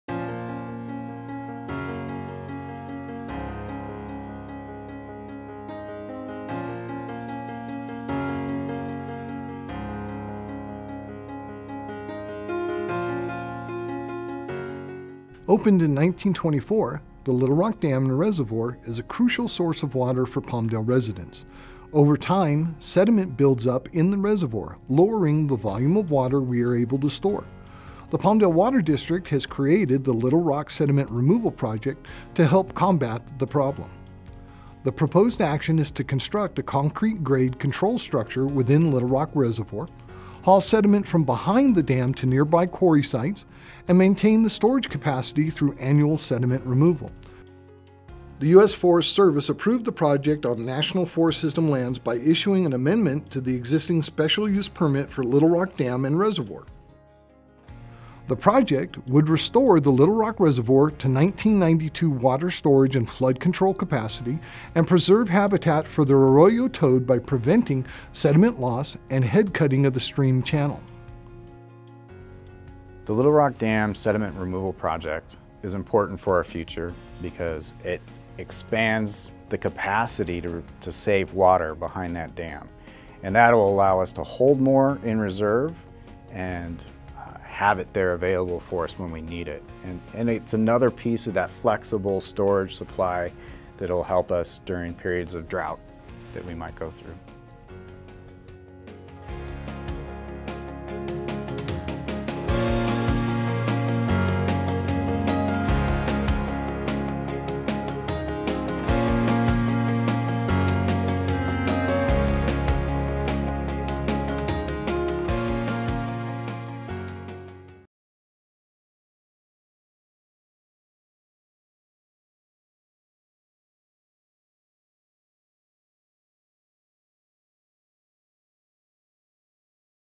Littlerock Reservoir Sediment Removal Project
To restore and preserve the reservoir's capacity, which has been substantially reduced over time by the accumulation of sediment behind Littlerock Reservoir during seasonal inflows, PWD will launch the Littlerock Reservoir Sediment Removal Project.